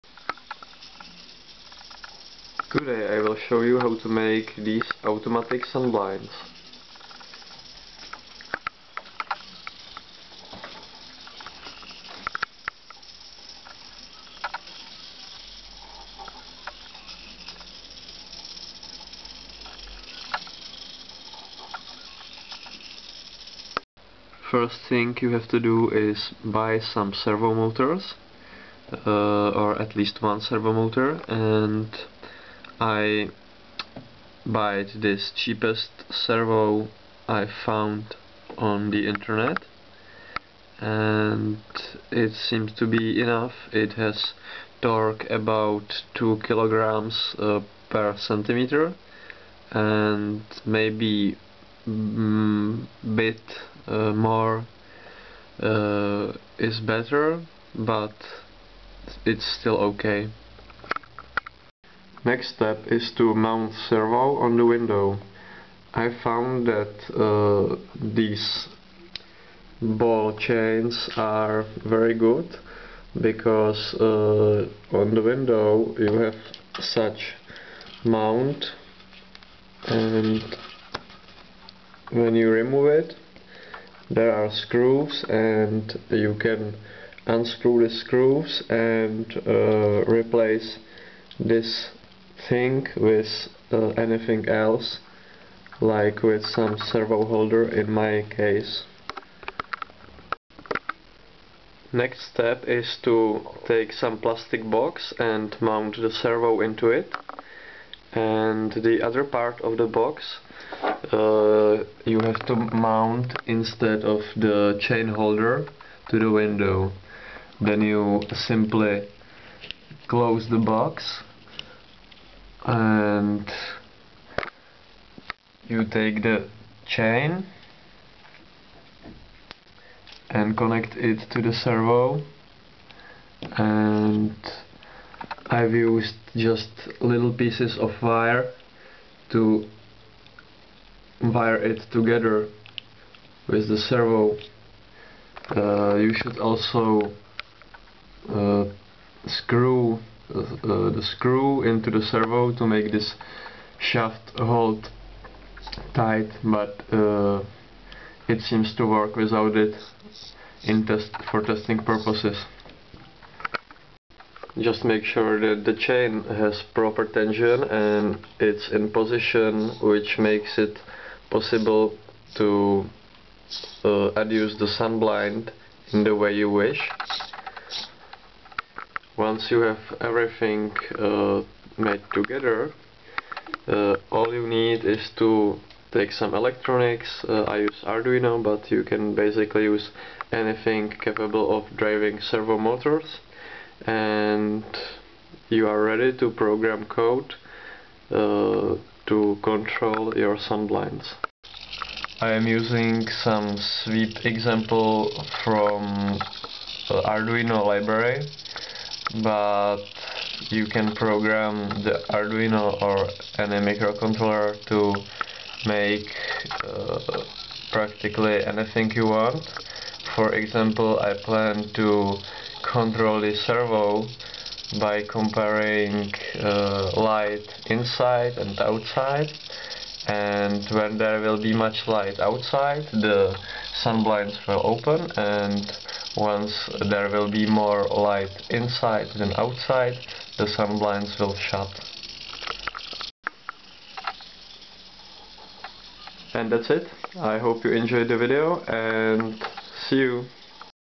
DIY Automatic Sunblinds Using $4 Servo Motor
This is how i've mounted $4 servo from DX to control my sunblinds.